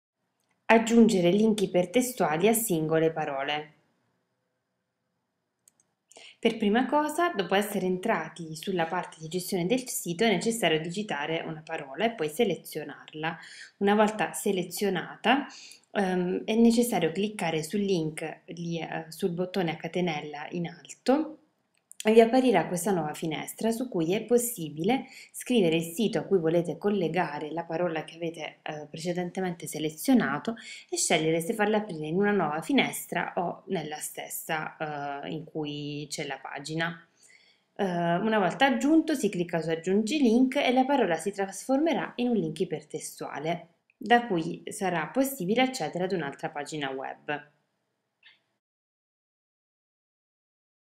Asoc tutorial - inserire collegamento ipertestuale su una parola
Come inserire un collegamento ipertestuale su una parola nel back end del sito